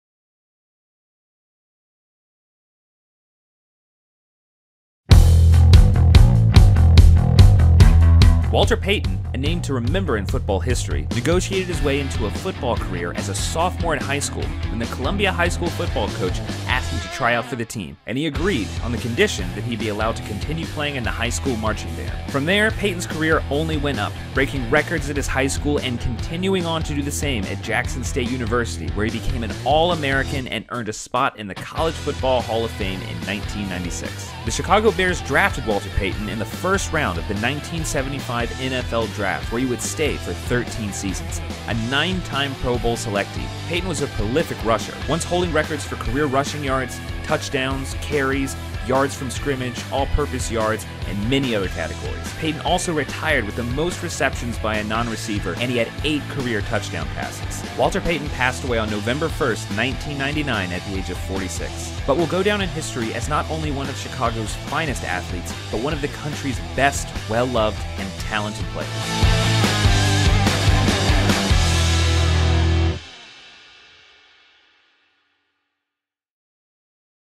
Walter Payton - Hall of Fame Induction Documentary
Sweetness personified, Walter Payton, the Bear who enchanted us all with an extraordinary 16,726 rushing yards over his career, embodying the relentless spirit of Chicago. Tune in to relive his inspiring legacy! 🐻🏈 WalterPayton Bears Chicago @ChicagoBears